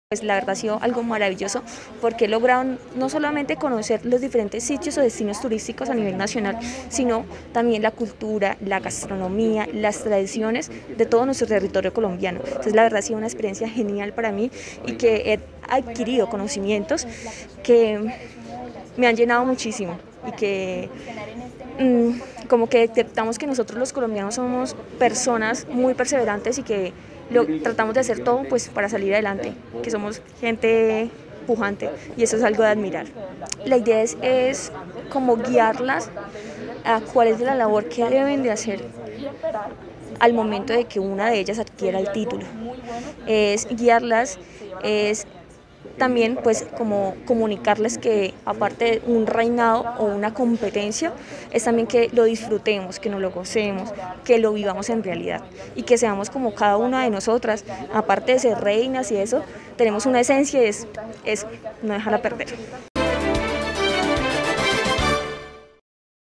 Daniela Angarita, actual reina Nacional del Turismo
También estuvo presente la actual reina nacional del turismo, Daniela Angarita, quien entregó un balance de la gestión y actividades que realizó a lo largo de sus dos años como embajadora del turismo nacional e internacional.